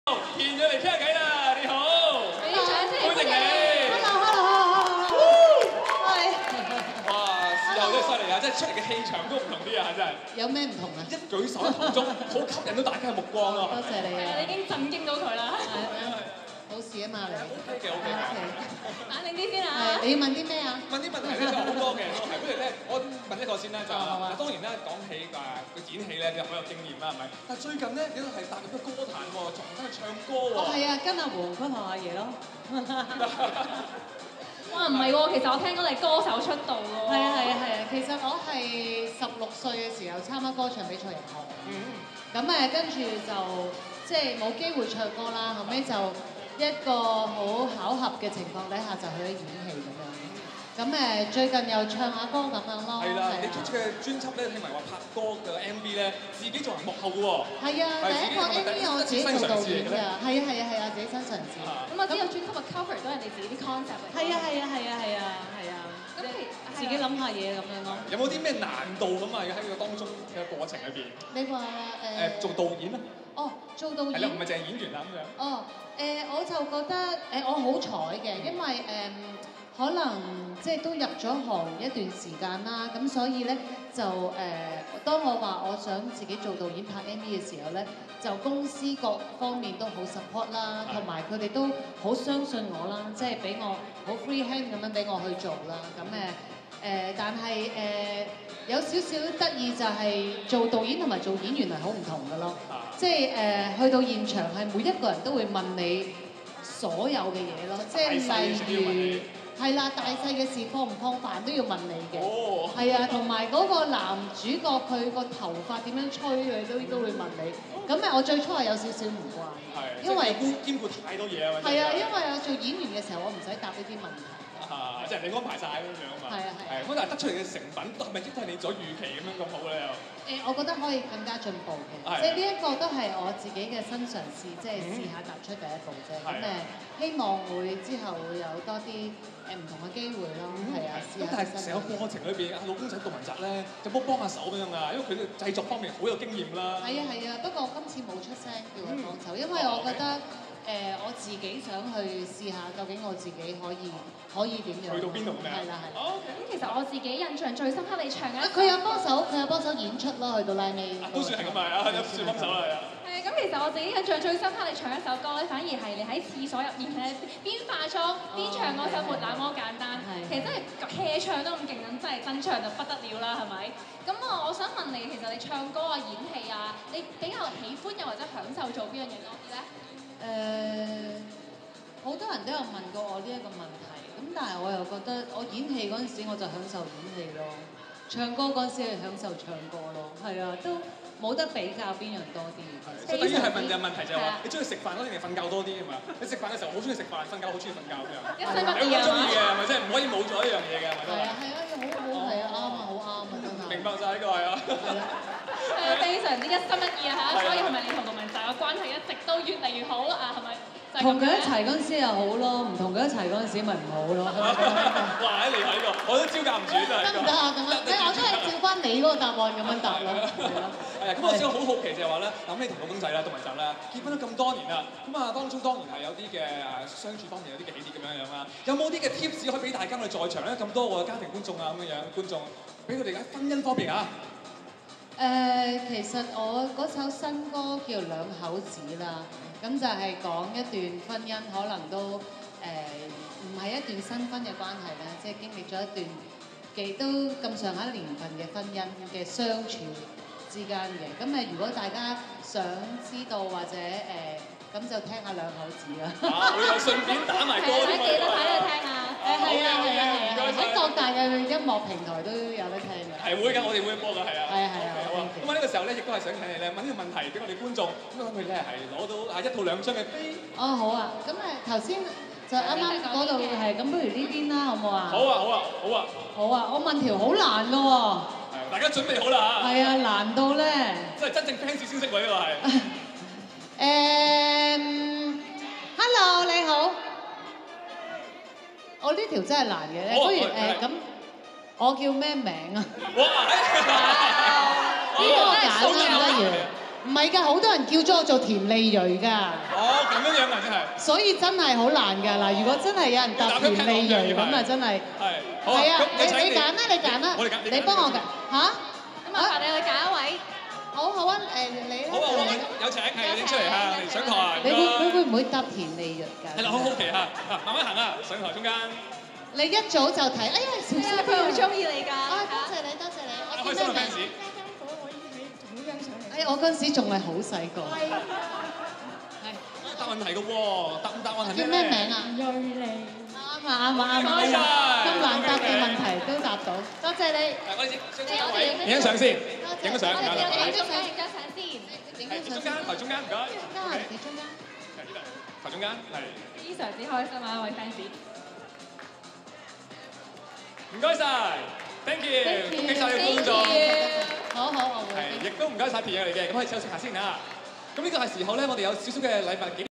【新聞發布會】2019魅力凝聚新時代 田蕊妮訪問 TVB Fairchild Fans Party Kristal Tin Interview
【新聞發布會】2019魅力凝聚新時代 TVB Fairchild Fans Party Press Conference

Wednesday April 17, 2019 | Aberdeen Centre, Richmond BC

Video by Ragin Ronin Productions Inc.